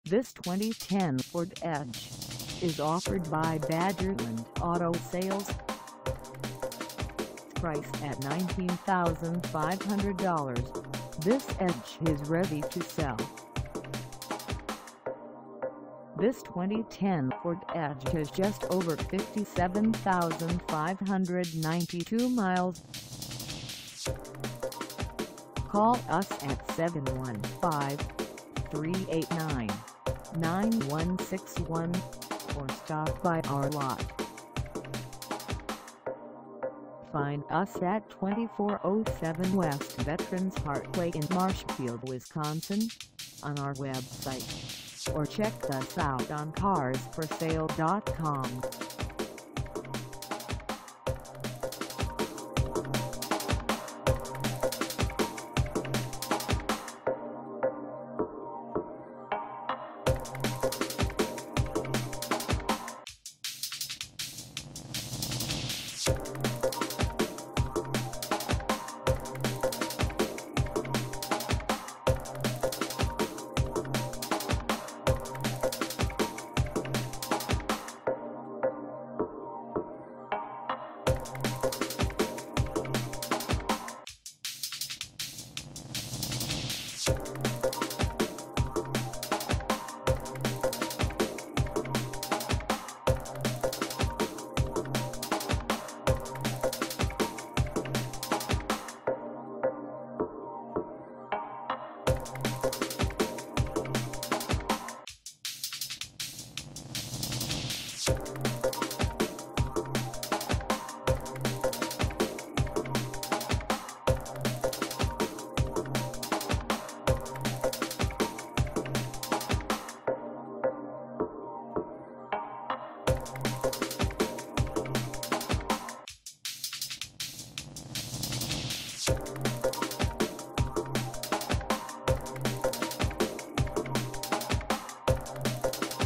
2010 Ford Edge SEL AWD - for sale in MARSHFIELD, WI 54449
BADGERLAND AUTO SALES

Come test drive this 2010 Ford Edge SEL AWD for sale in MARSHFIELD, WI.

Black Beauty 2010 Ford Edge SEL AWD with 57k miles! ONE OWNER! VIN: 2FMDK4JC1ABB70763. Engine: 3.5 L., 6 Cyl. Transmission: 6-Speed Automatic. This is a super clean, 5 passenger, fully loaded, AWD SUV. Options include dual power and heated leather seats, dual climate control, Sync by MicroSoft, power locks, power windows, power mirrors, power folding rear seats for extra cargo space, tilt steering, telescopic steering, air, and cruise. Stop at Badgerland Auto Sales to test drive this perfect winter driving vehicle today, or call for more information. Ask for Keith or Dawn. We look forward to seeing you!

Badgerland Auto Sales
Your trusted, family owned dealer since 1987!